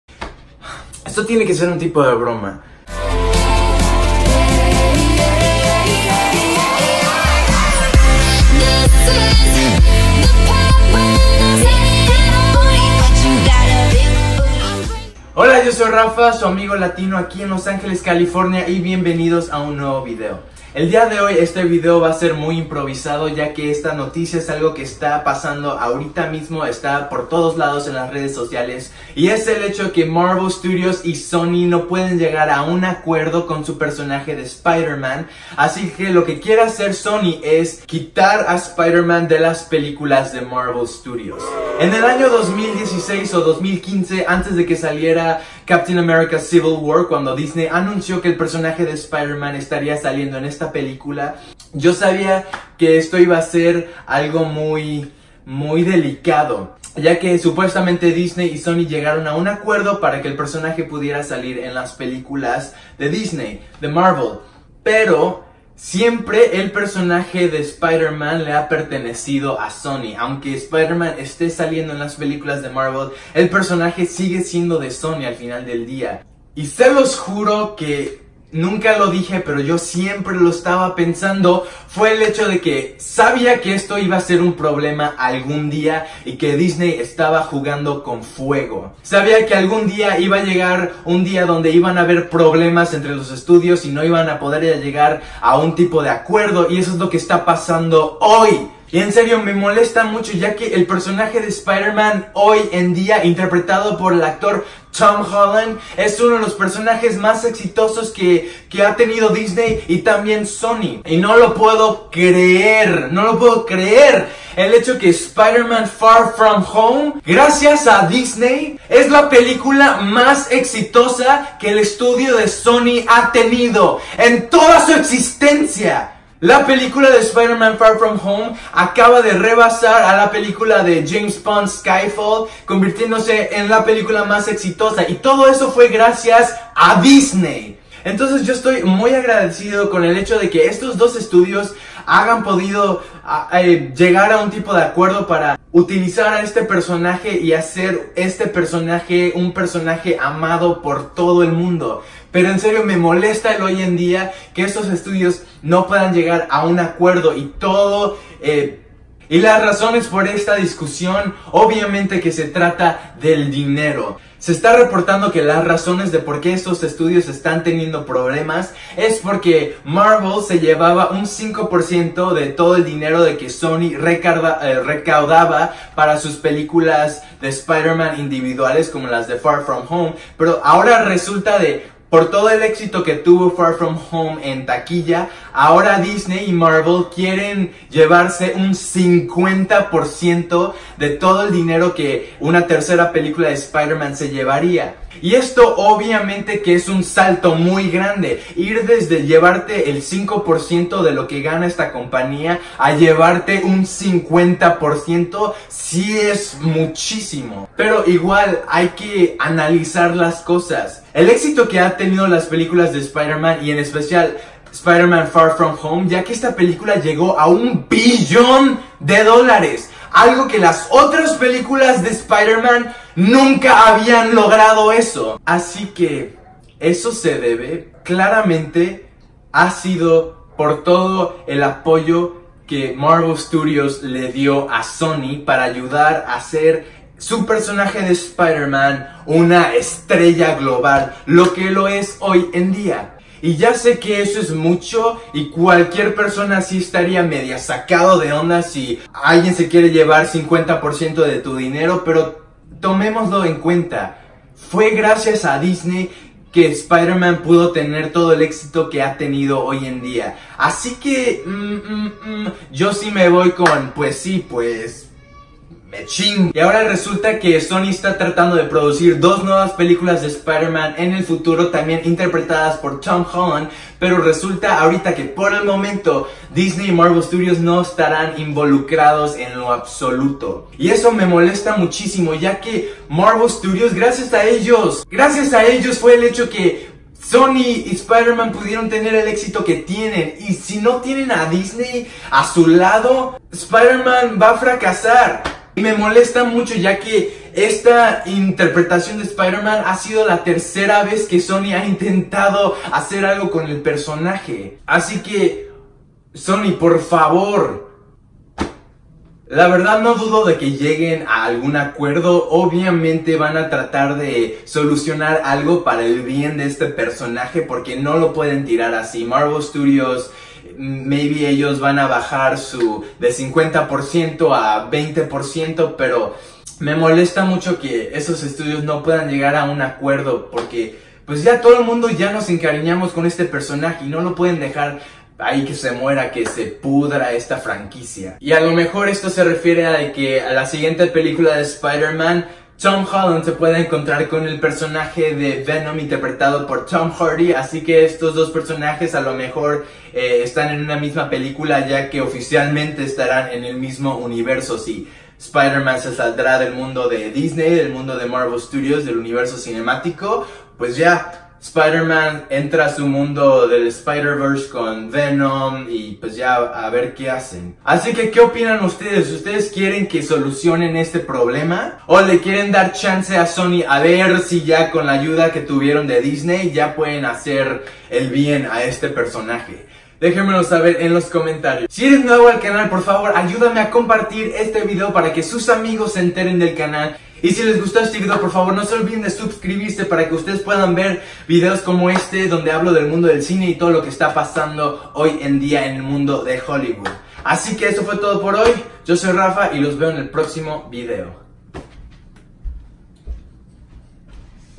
SPIDERMAN FUERA DE MARVEL STUDIOS!? / RAFA
Holaaa!!! En este video les explico un poco sobre lo que esta pasando con el personaje de Spiderman!! Diganme que opinan ustedes!